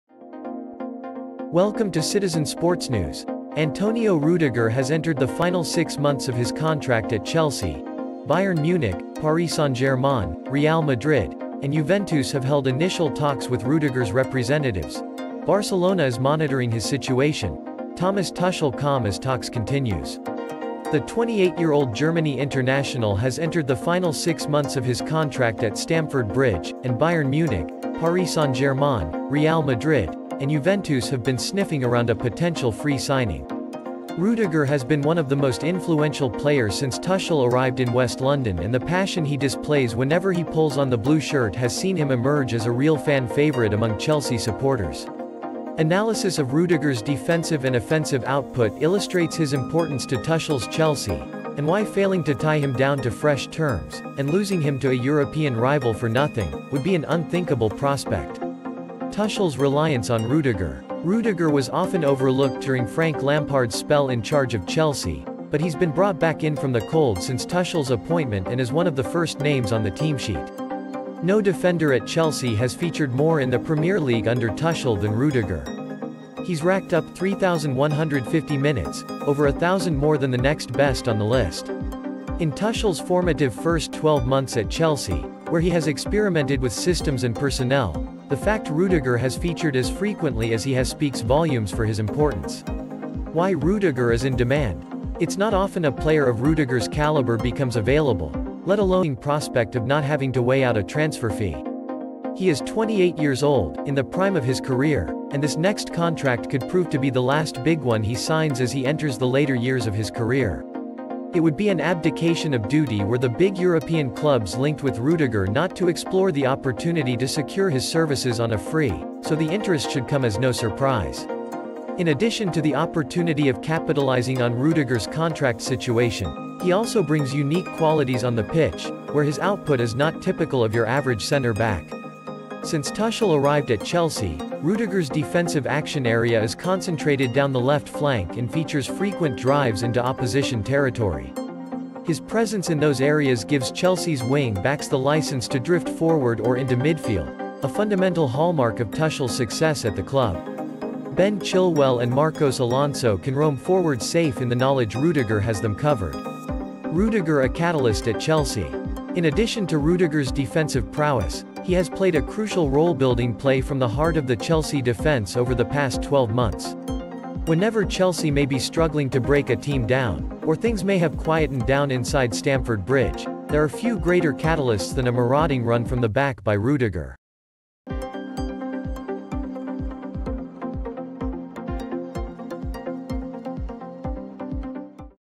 Why Tuchel's Chelsea Must "TIE DOWN" Antonio Rudiger In Fresh Contract Terms Than Losing Him
Why Tuchel's Chelsea Must "TIE DOWN" Antonio Rudiger In Fresh Contract Terms Than Losing Him. Antonio Rudiger has entered the final six months of his contract at Chelsea; Bayern Munich, Paris Saint-Germain, Real Madrid, and Juventus have held initial talks with Rudiger's representatives; via @YouTube

 Barcelona is monitoring his situation; Thomas Tuchel calm as talks continues.
The 28-year-old Germany international has entered the final six months of his contract at Stamford Bridge, and Bayern Munich, Paris Saint-Germain, Real Madrid, and Juventus have been sniffing around a potential free signing.
Rudiger has been one of the most influential players since Tuchel arrived in west London and the passion he displays whenever he pulls on the blue shirt has seen him emerge as a real fan favourite among Chelsea supporters.
Analysis of Rudiger's defensive and offensive output illustrates his importance to Tuchel's Chelsea, and why failing to tie him down to fresh terms, and losing him to a European rival for nothing, would be an unthinkable prospect.
Tuchel's reliance on Rudiger.
Rudiger was often overlooked during Frank Lampard's spell in charge of Chelsea, but he's been brought back in from the cold since Tuchel's appointment and is one of the first names on the teamsheet.
No defender at Chelsea has featured more in the Premier League under Tuchel than Rudiger. He's racked up 3,150 minutes - over a thousand more than the next best on the list.
In Tuchel's formative first 12 months at Chelsea, where he has experimented with systems and personnel, the fact Rudiger has featured as frequently as he has speaks volumes for his importance.
Why Rudiger is in demand.
It's not often a player of Rudiger's calibre becomes available, let alone on the enticing prospect of not having to weigh out a transfer fee.
He is 28 years old, in the prime of his career, and this next contract could prove to be the last big one he signs as he enters the later years of his career.
It would be an abdication of duty were the big European clubs linked with Rudiger not to explore the opportunity to secure his services on a free, so the interest should come as no surprise.
In addition to the opportunity of capitalising on Rudiger's contract situation, he also brings unique qualities on the pitch, where his output is not typical of your average centre-back.
Since Tuchel arrived at Chelsea, Rudiger's defensive action area is concentrated down the left flank and features frequent drives into opposition territory.
His presence in those areas gives Chelsea's wing-backs the licence to drift forward or into midfield, a fundamental hallmark of Tuchel's success at the club. Ben Chilwell and Marcos Alonso can roam forward safe in the knowledge Rudiger has them covered.

Rudiger a catalyst at Chelsea.
In addition to Rudiger's defensive prowess, he has played a crucial role building play from the heart of the Chelsea defence over the past 12 months.
Whenever Chelsea may be struggling to break a team down, or things may have quietened down inside Stamford Bridge, there are few greater catalysts than a marauding run from the back by Rudiger.